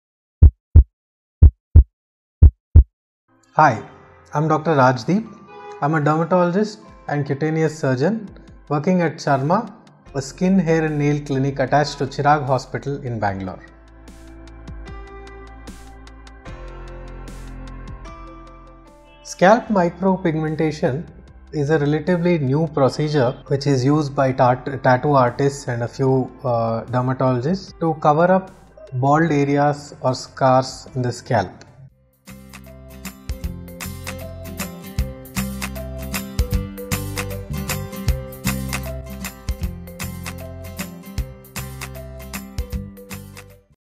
What is Scalp Micro Pigmentation? - Dr. Rajdeep Mysore
Scalp Micro Pigmentation is a relatively new procedure which is used by tattoo artist and a few dermatologists to cover up bald areas or scars in the scalp.